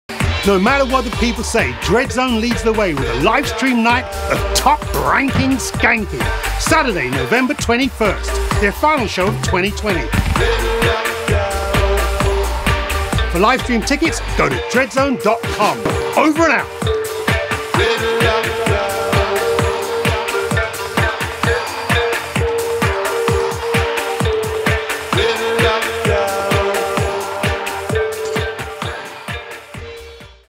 LOCKDOWN LIVESTREAM Saturday 21 November 2020
STREAMING LIVE FROM A STUDIO IN LONDON
This is a one-off, high definition, multi-camera stream. Following the music there is an opportunity for a strictly limited number of fans to zoom in and join members of band for an ‘Aftershow’ where DJ, film-maker and musician Don Letts will be there in person hosting a Q&A and chatting to us.

General Admission – link will be emailed to you to watch online in a browser on phone, computer, other device or connect to TV
Virtual Front Row – strictly limited number via Zoom to join an audience the band can play to, and interact with, on screens in front of them in the studio

The event will be produced by Grammy nominated Rupert Style (Nick Cave, Rhianna) and mixed live by Dreadzone co-founder and producer Tim Bran (London Grammar, La Roux).